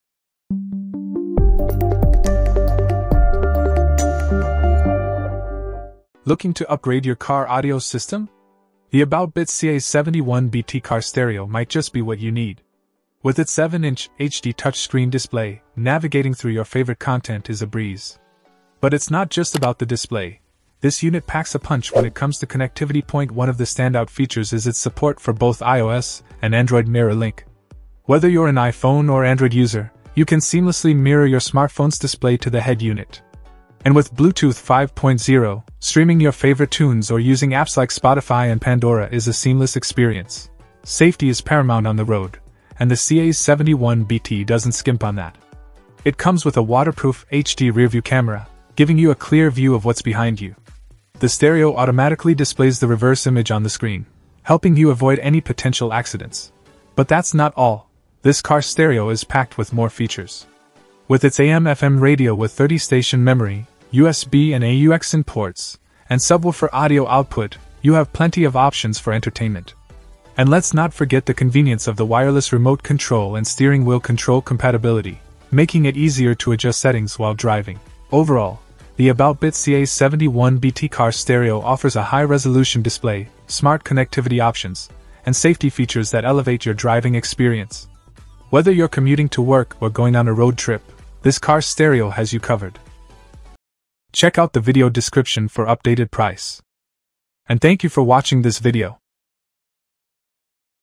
aboutBit CA-71BT Car Stereo: Elevate Your Driving Experience with Smart Connectivity
0:00 Intro
0:06 Review

Things that we mentioned in this video:
Bluetooth Double Din Car : B08975BZPP